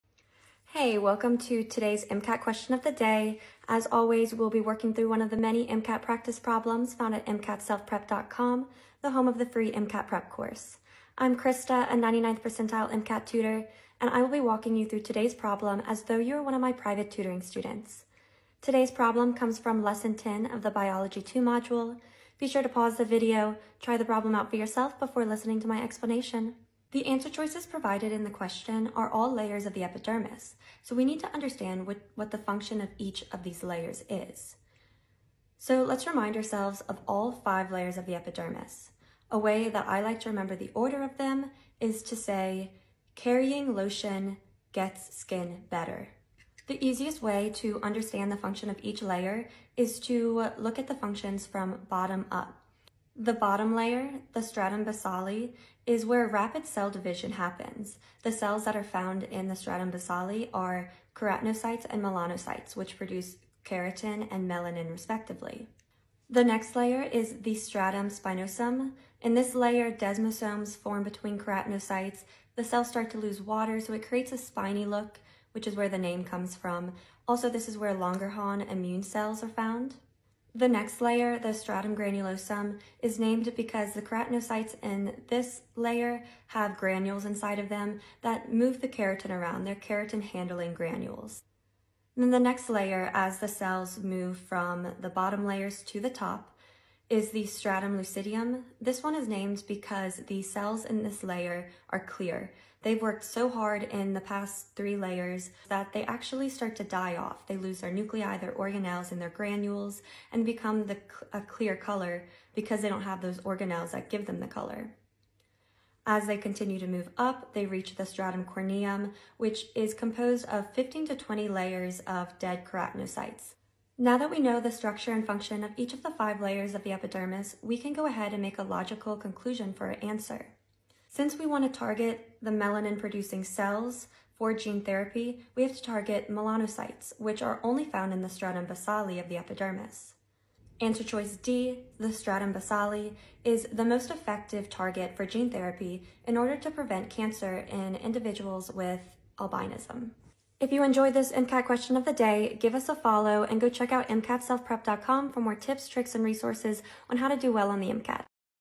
MCAT Question of the Day - Layers of Epidermis
There are five layers of the epidermis. Do you know what the purpose of each layer is? In this video, we break down the structure and function of each layer of the epidermis using a real-world example: the development of gene therapy for albinism.